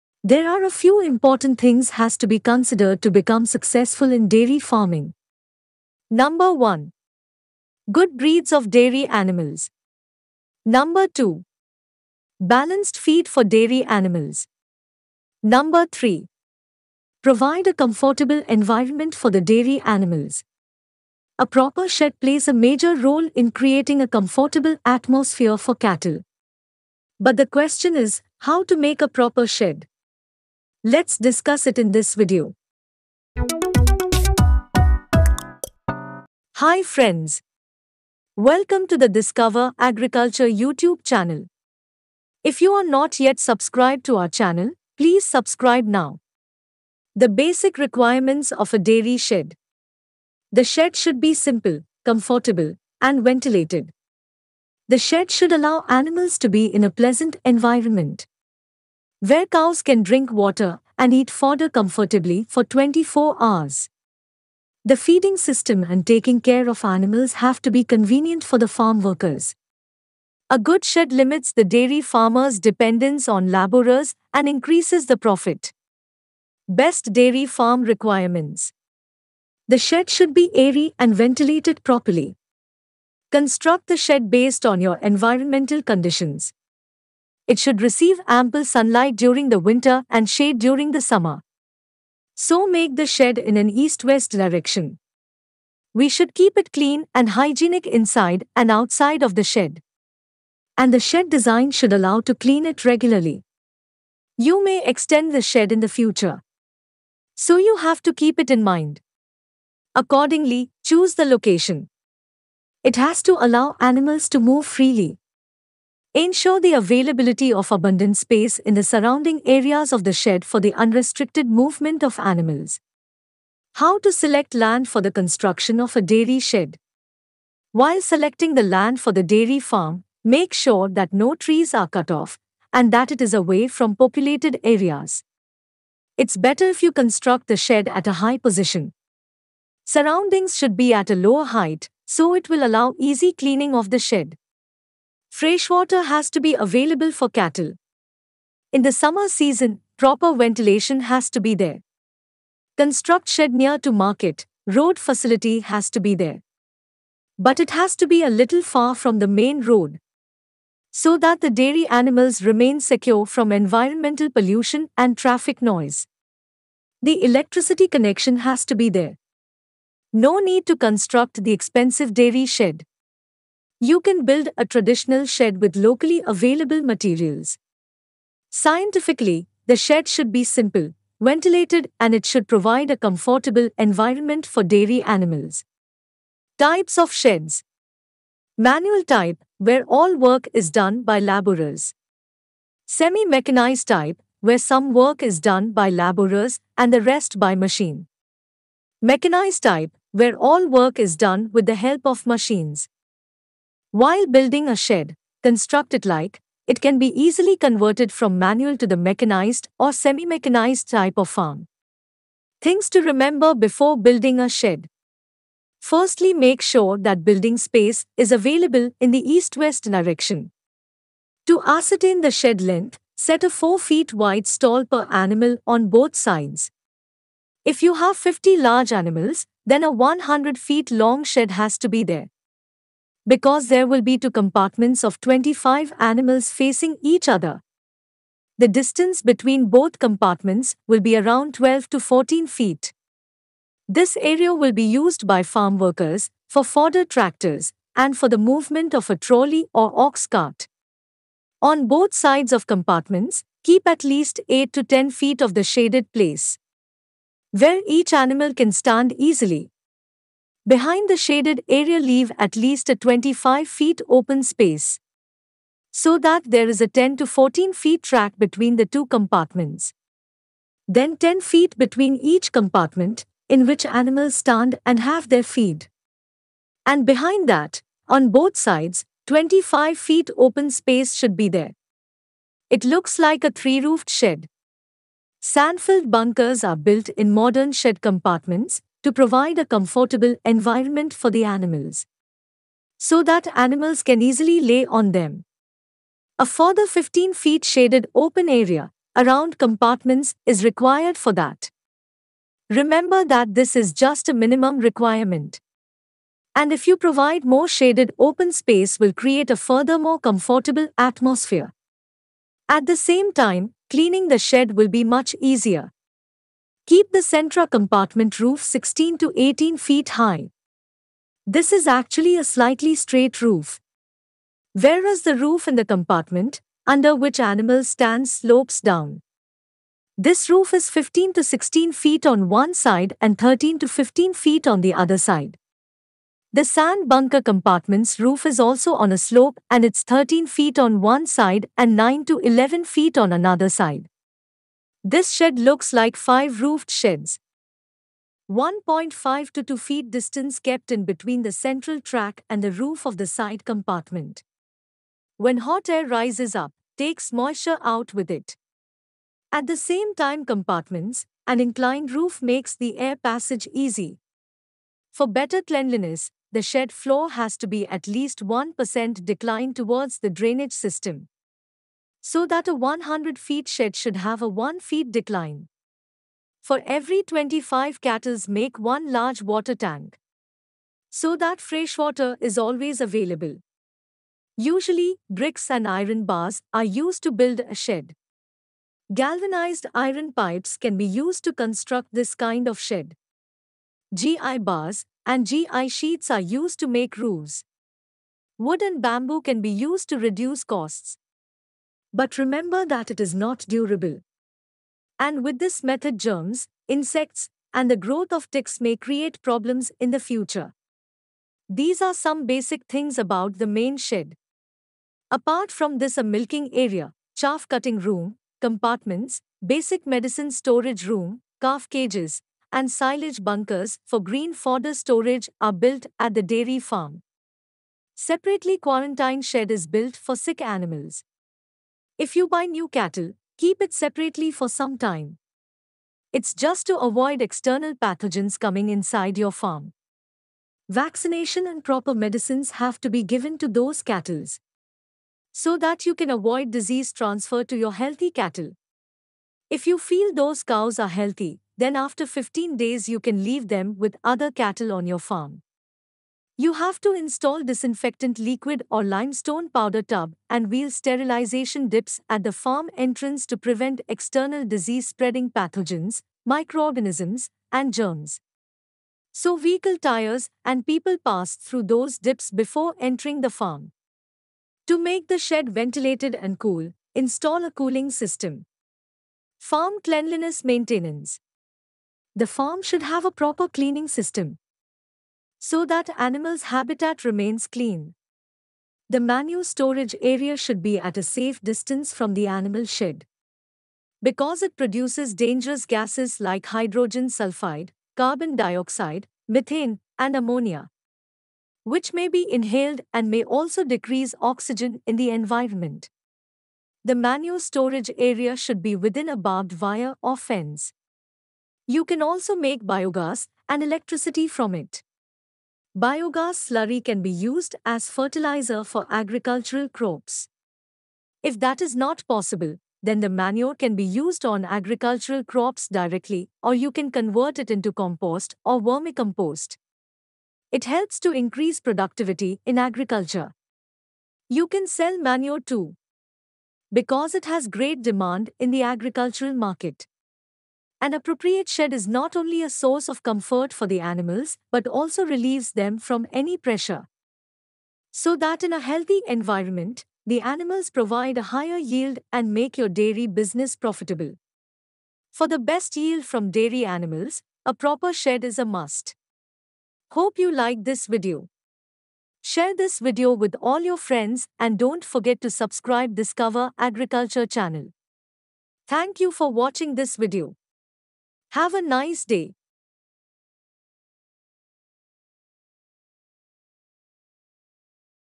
Dairy Shed Design, Construction Plan Information | Dairy Cow Farming
Dairy farming is a form of agricultural practice, where livestock for milk production is raised. In India, 2.28 million metric tonnes of milk are produced each year, but the demand for milk is 12.52 million metric tonnes per year, i.e. shortage of milk in a year is 10.24 million metric tonnes.

The milk produced by cows is very important in day-to-day life. It is one of the most profitable forms of business, but the choice of the breed should be proper to yield better results. Farmers should learn more techniques about dairy management, proper breeding, methods to prevent diseases, primary care, balanced feed management, and many more modern methods for good production.

Video Credits: USAID Dairy Project & SHUMAILA JAMIL

0:00 How to become Successful in Dairy farming Business
0:32 Introduction
0:47 The Basic Requirements of a Dairy Shed
1:16 Best Dairy Farm Requirements
2:03 How to Select Land for the Construction of a Dairy Shed
3:06 Types of Dairy Sheds
3:33 Things to Remember before Building a Shed
8:00 Farm Cleanliness Maintenance
9:20 Bottom line